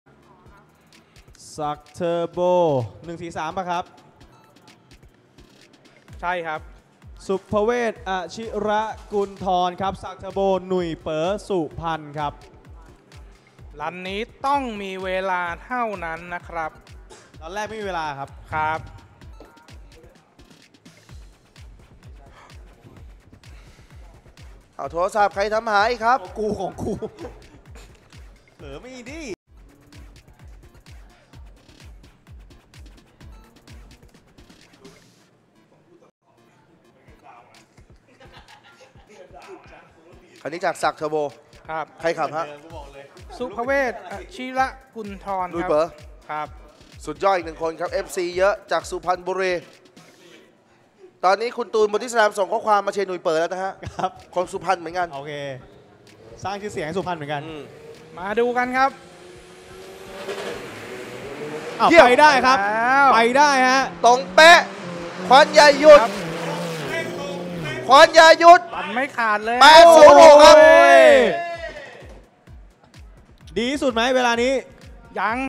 FINAL RACE 2 : 143 ศุภเวช อชิระกุนทร / ศักดิ์เทอร์โบ & หนุ่ย-เป๋อ สุพรรณ / SOUPED UP 2018
LEO Presents ECU=Shop Souped Up Thailand Records 2018 - Final 2nd Run
PRO TRUCK